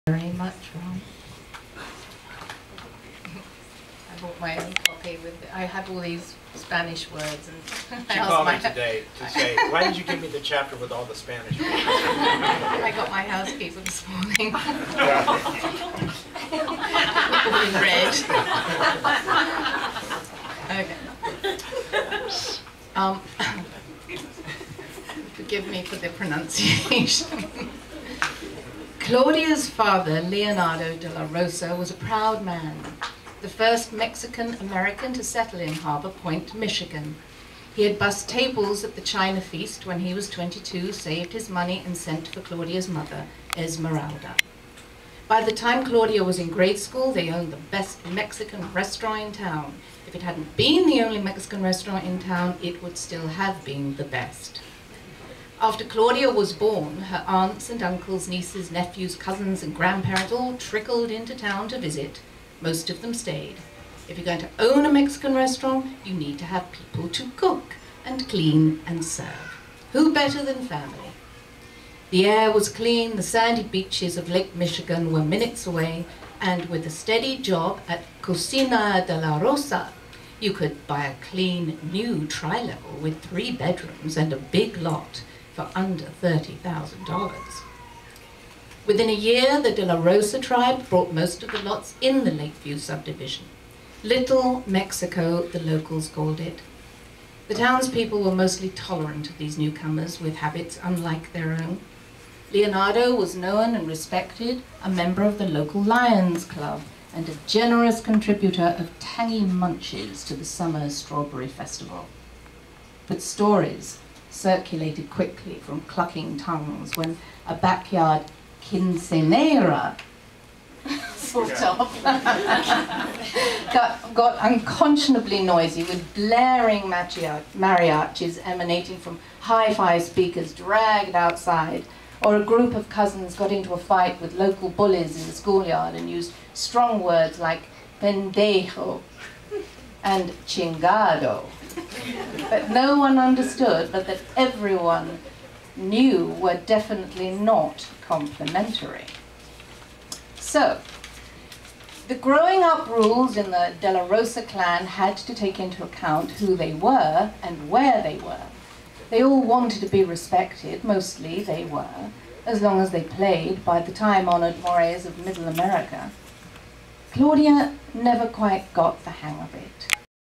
Lesley-Anne Down reads from "Soul Mate"
Ronn Moss, Lesli Kay, Lesley-Anne Down and Joe Mascolo joined B&B producer Ron Weaver at Barnes & Noble to read excerpts from his new book, "Soul Mate" to a star studded audience. Here, Down gives backstory on one of the main characters.